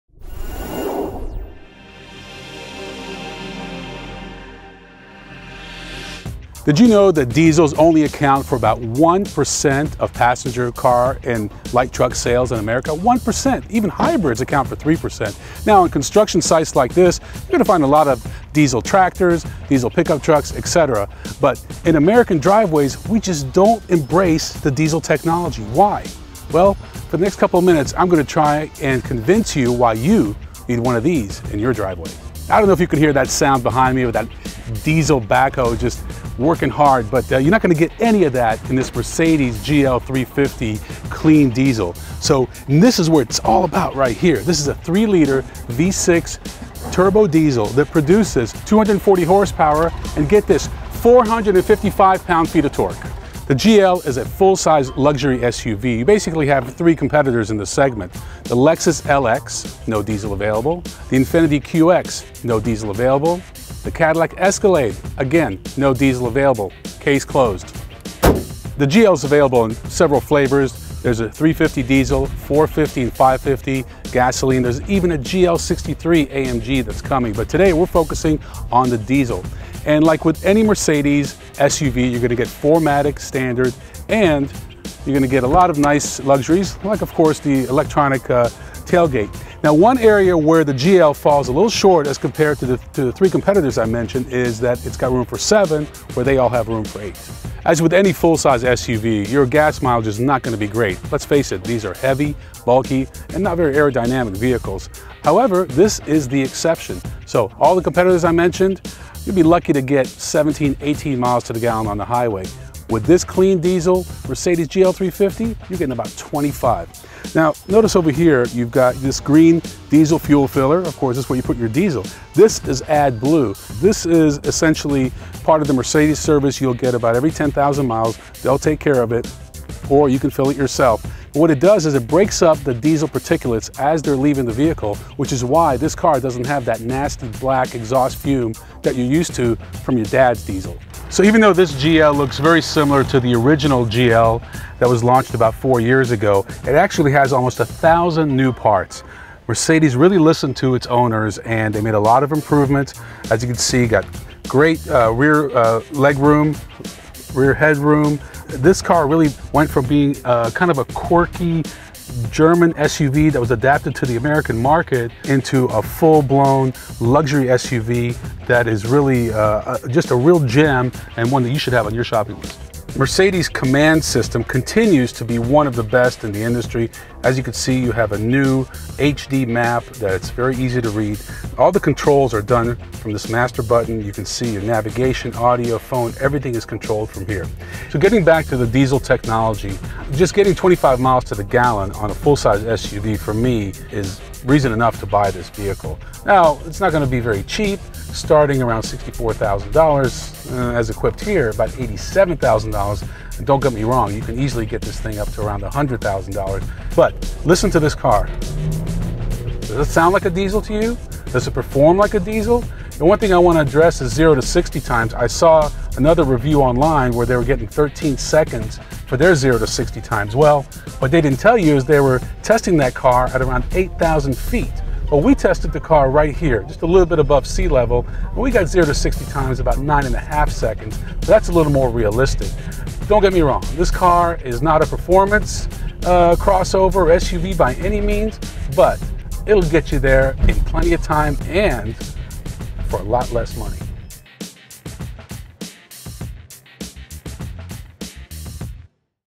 2013 Mercedes-Benz GL350 BlueTec Diesel Test Drive & Luxury SUV Video Review
The 2013 Mercedes GL-Class is a full sized luxury SUV. There are 3 different trim levels that include the GL550, GL450, and GL350. The original GL-Class was introduced to the U.S. as a 2007 model and not too much has changed since that time. The GL is a 5-door 7-passenger SUV. Similar luxury SUVs in this class include the Audi Q7, Infiniti QX, Lexus LX, and the Cadillac Escalade; only one of which offers a diesel model. The starting price is $62,400 for the GL350 with the top of the line GL550 model costing $86,900.

Did you know that diesels only account for 1% of all passenger car and truck sales in the U.S. while hybrids account for more than 3%?  For some reason Americans just do not embrace the diesel technology. The 2013 Mercedes GL350 BlueTec is a good reason why you should look into diesel powered vehicles. The engine is a 3.0 liter turbodiesel that makes 240 horsepower and 455 lb-ft. of torque. Most full sized SUVs have a hard time breaking the 20 MPG marker since they are heavy, bulky, and not very aerodynamic. But the 2013 Mercedes GL350 BlueTec averages 19 MPG in the city and 26 MPG on the highway. The transmission is a 7-speed automatic. Like with any Mercedes SUV, 4MATIC AWD is standard as are a bunch of features and luxury amenities.

Even though this GL looks like the original 2007 model, there are actually over 1000 new parts. Mercedes really listened to its owners and they made a lot of improvements over the past few years. There is a ton of rear passenger leg and head room. The GL-Class went from being a quirky German made SUV that was adapted for the American market to a full-blown luxury SUV that is real gem.